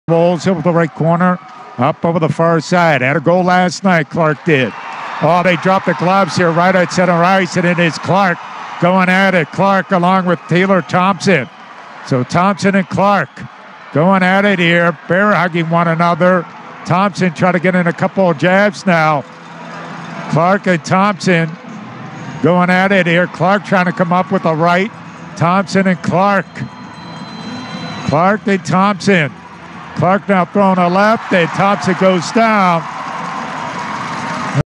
Aaron Clarke vs Tayler Thompson 102216
Aaron Clarke drops the mitts with Tayler Thomson early into the opening period on 10/22/16